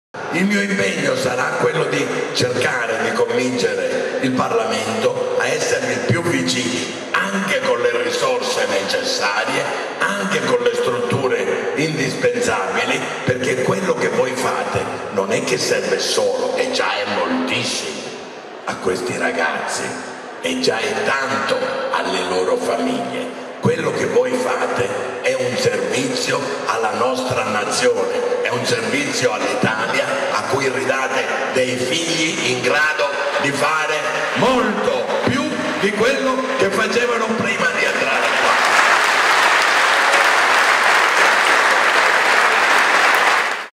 IL PRESIDENTE LA RUSSA IN VISITA ALLA COMUNITÀ DI RECUPERO DI POZZOLENGO
(Agenzia Italia News) IL PRESIDENTE DEL SENATO IGNAZIO LA RUSSA IN VISITA ALLA COMUNITÀ DI RECUPERO DI POZZOLENGO